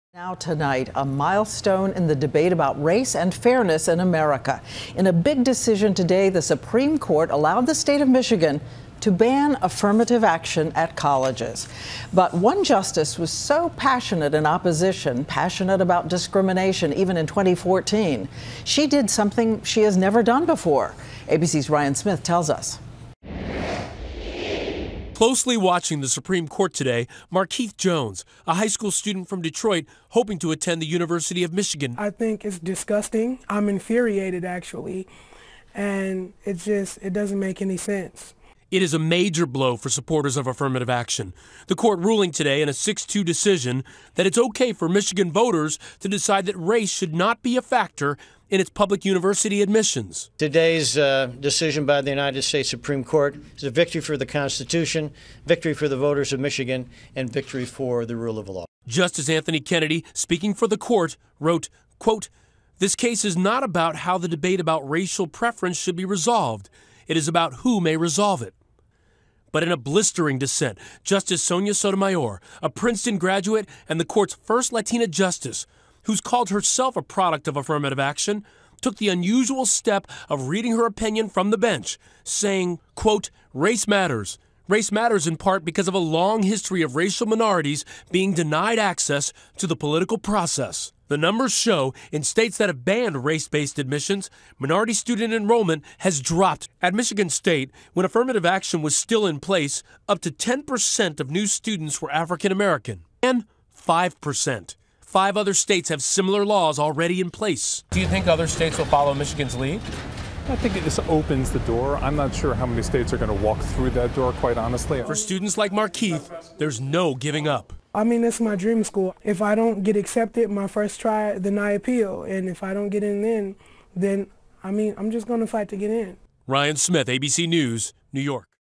Michigan Ban on College Affirmative Action Stand
A divided Supreme Court upheld the state's restriction on race-influenced admissions policies.